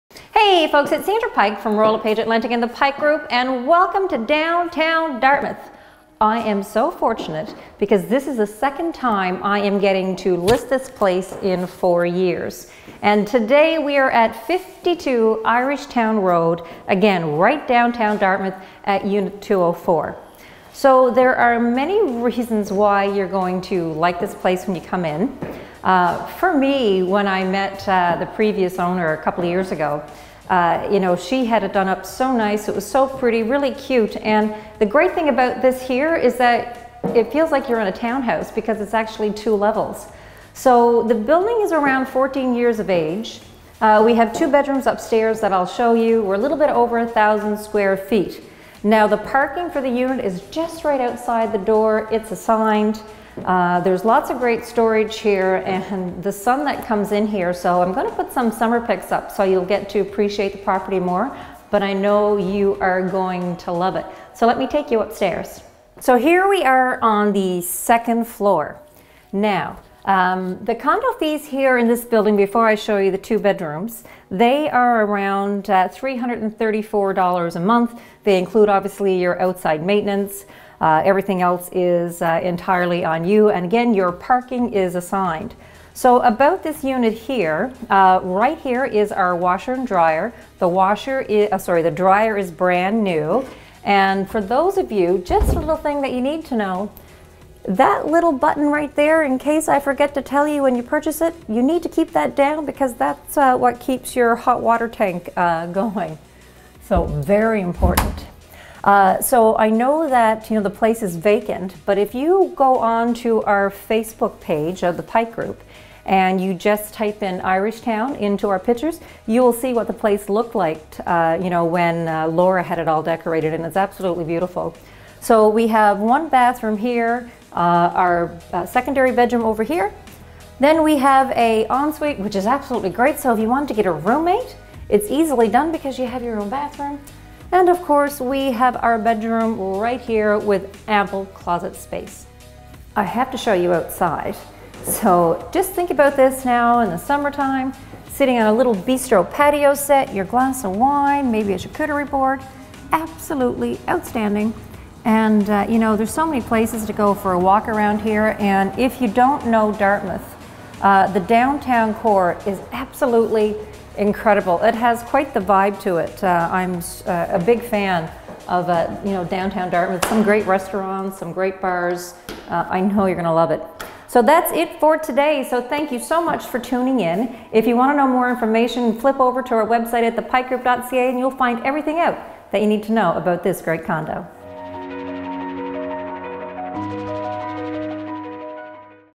Darmouth Real Estate : Unit 204 52 Irishtown Road
If you're looking for a condo close to everything we have the perfect place for you!

Located in Dartmouth's downtown area this townhouse style condo is surrounded by shopping, dining and culture. Convenient location allows ease of access to the ferry to Halifax or enjoy a stroll along the Canal to Sullivan's Pond.
This unit offers a homelike setting over 2 levels. The main living area is on the second floor and features an open floor plan with patio off of the living room.
The master bedroom with ensuite, as well as second bedroom and separate bath are located on the top floor. You will enjoy this quiet building that is also pet friendly!